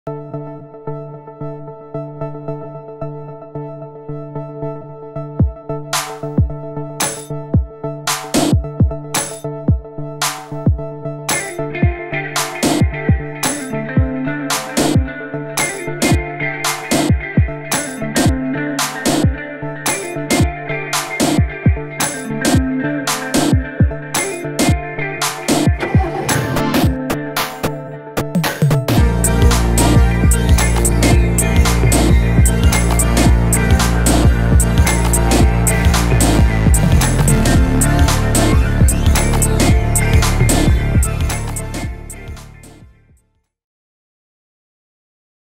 Lotus Elise Sport
[WERBUNG] Schmackofatzo war wieder für eine Video-Produktion unterwegs! Diesmal mit einem Lotus Elise Sport.

Video & Schnitt: Morteza Yazdi